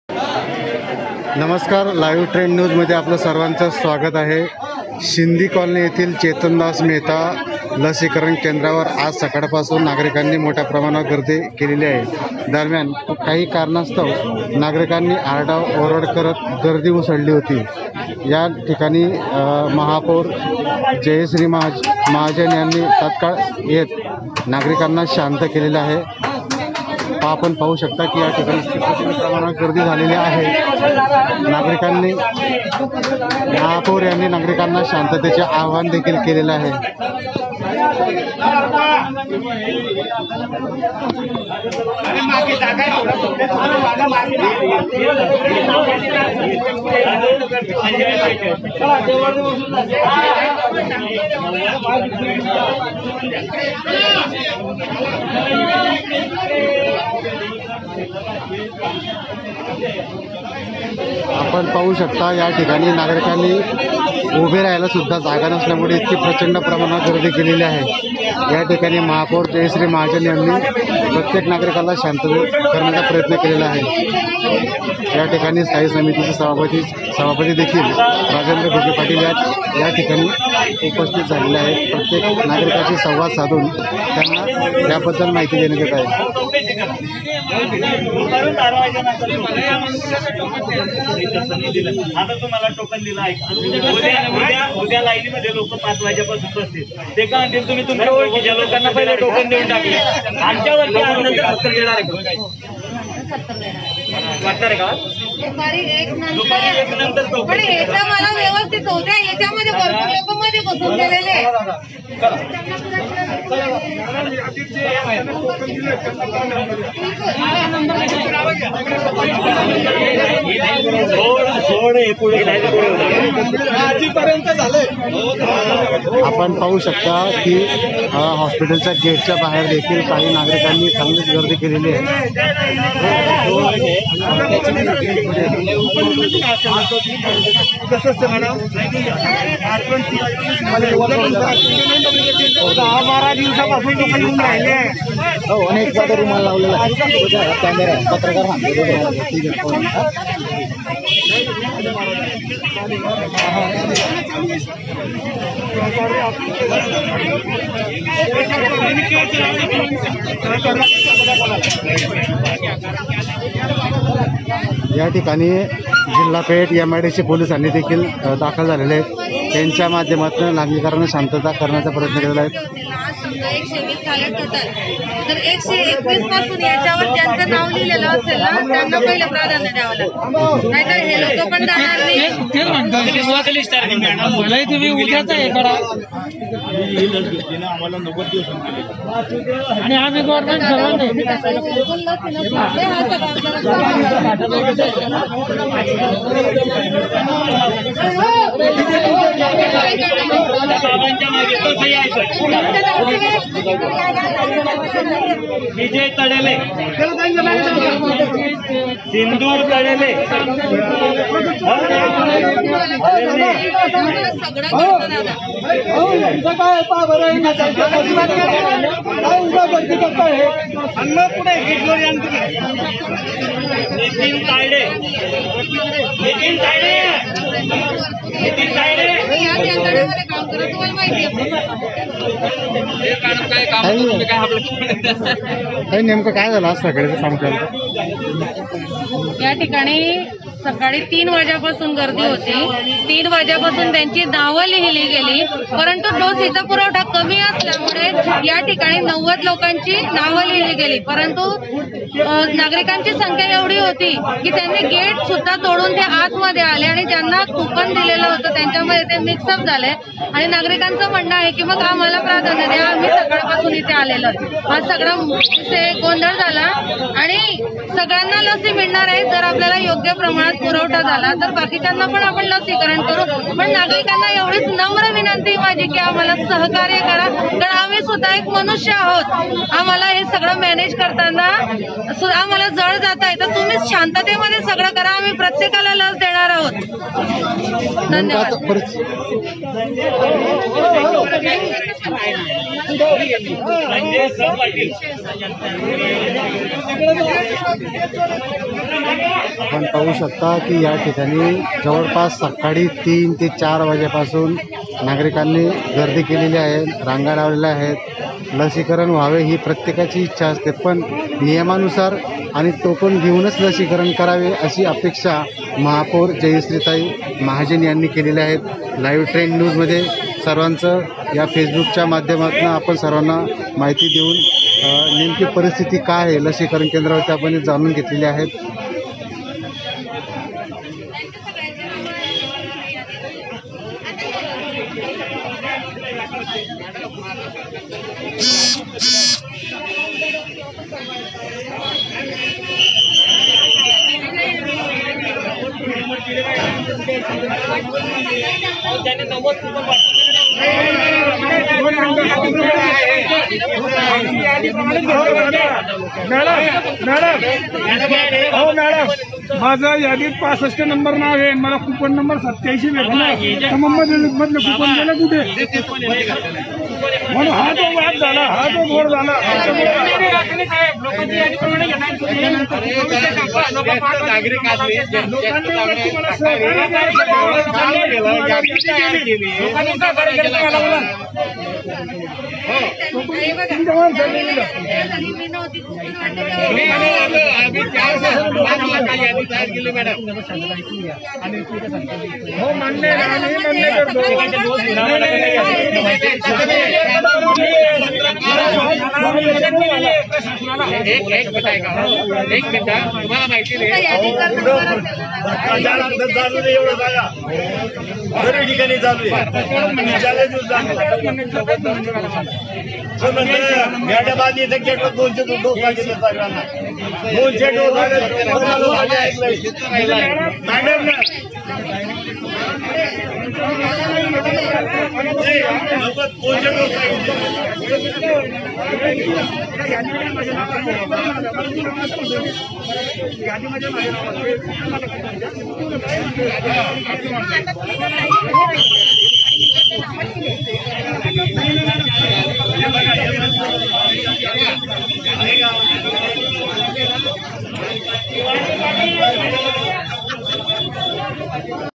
जळगावात लसीचे नियोजन कोलमडले; महापौरांची केंद्रावर धाव
जळगाव जितेंद्र कोतवाल । आज पहाटे तीन वाजेपासून येथील चेतनदास मेहता रूग्णालयातील लसीकरण केंद्रावर नागरिकांनी रांगा लावल्या. मात्र, येथे आज लसींचा पुरेसा साठा नसल्यामुळे लोकांना लस मिळणार नसल्याने याची संतप्त प्रतिक्रिया उमटली. महापौर सौ. जयश्री सुनील महाजन यांनी या लसीकरण केंद्रावर जाऊन नागरिकांनी समजूत घालण्याचा प्रयत्न केला. मात्र लोक ऐकण्याच्या मनस्थितीत नसल्याचे दिसून आले आहे.